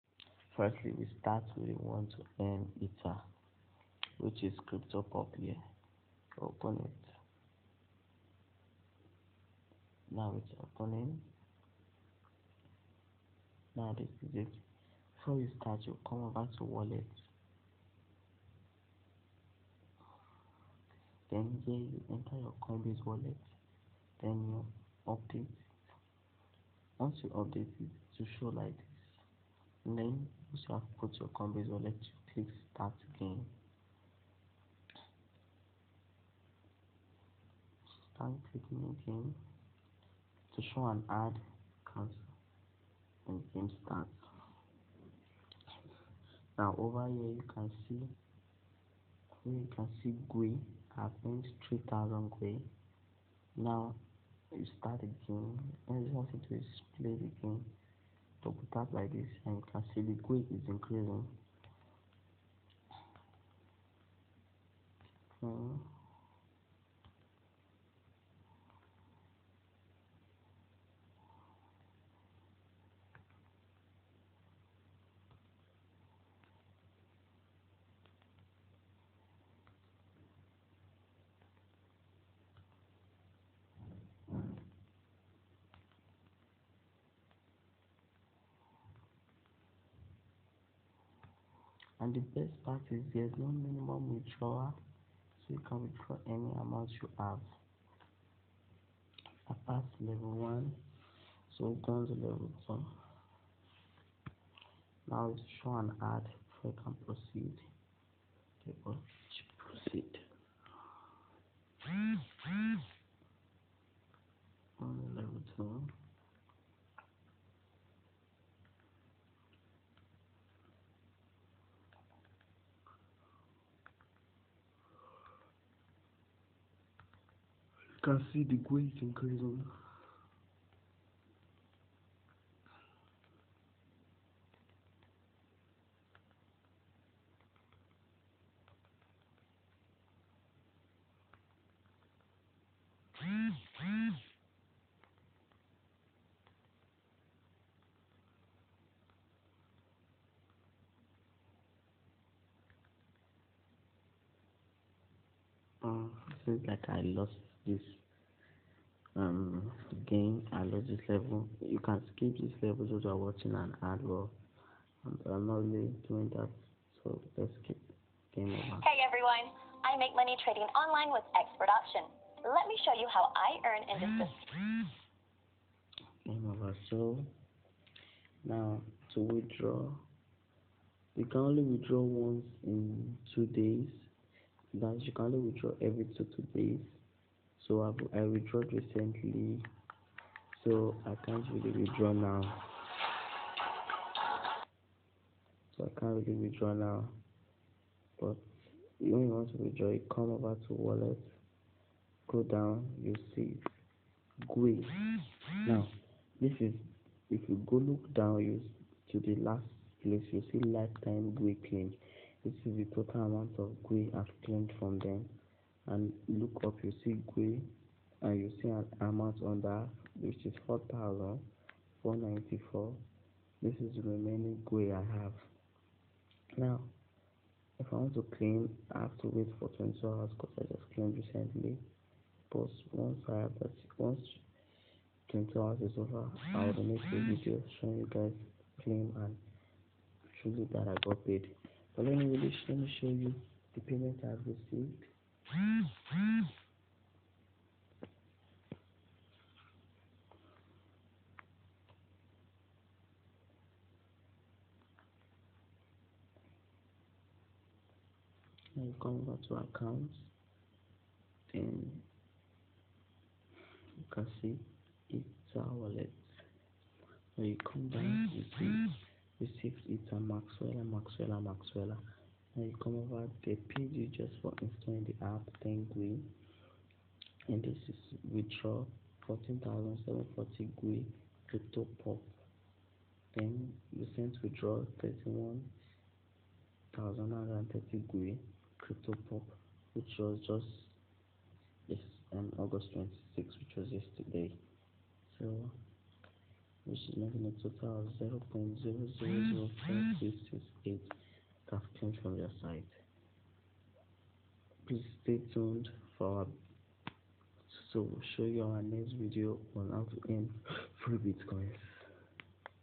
Earning Free Unlimited Ethereum Easily
This is a video about an app to earn free unlimited Ethereum easily without minimum withdrawal, with proof of payment

Just head on to Play store and search for CryptoPop, install and enjoy.